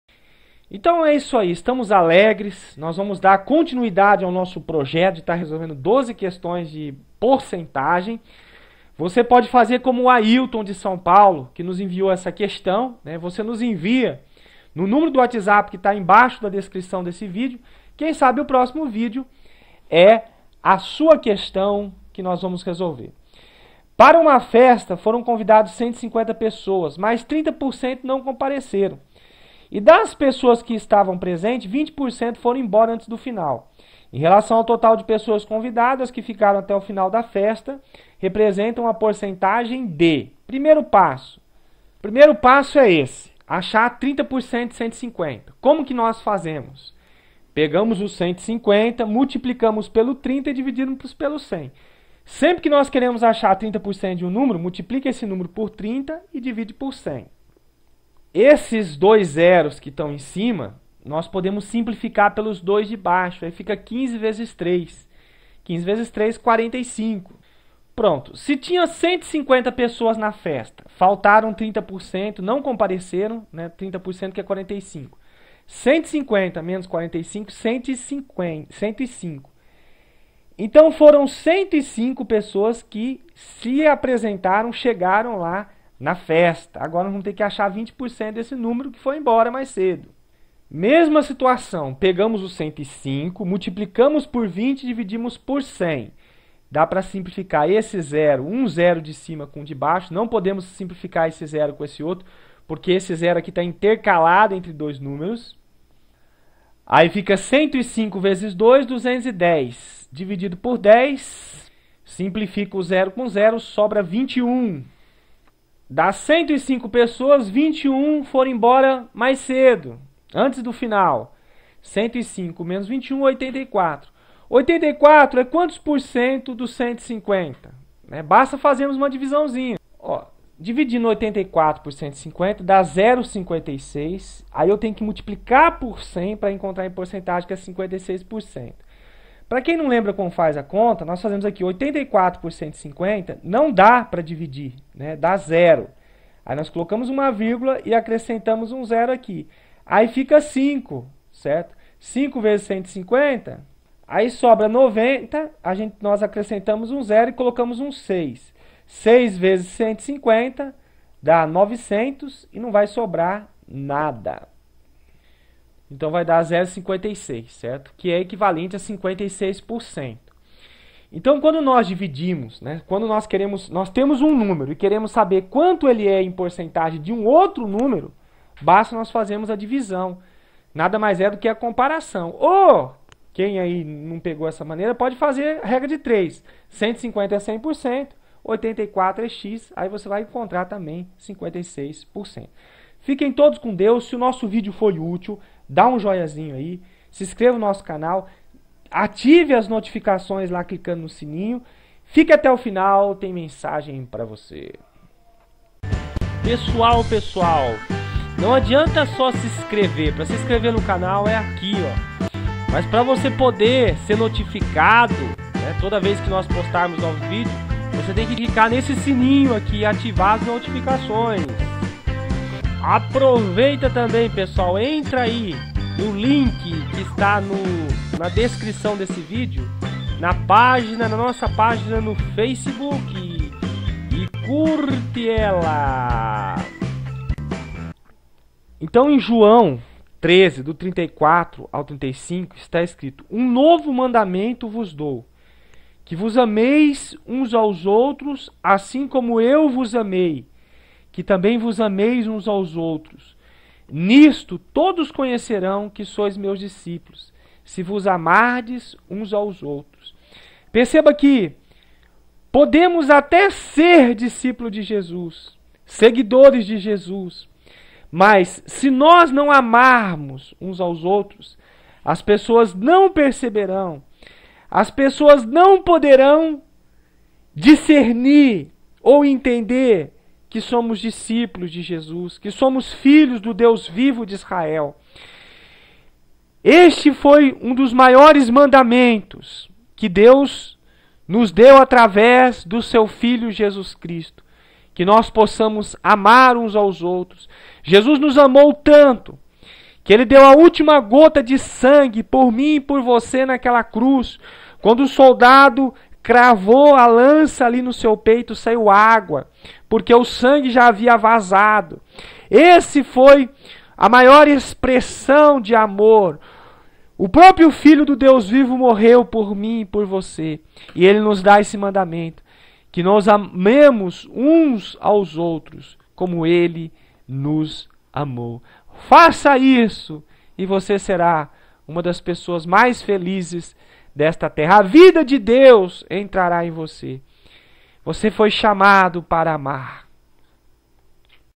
QUESTÃO 06 - MATEMÁTICA E PORCENTAGEM PARA CONCURSOS - P12*06
curtir pagina no facebook

Para uma festa foram convidadas 150 pessoas, mas 30% não compareceram e, das pessoas que estavam presentes, 20% foram embora antes do final. Em relação ao total de pessoas convidadas, as que ficaram até o final da festa representam uma porcentagem de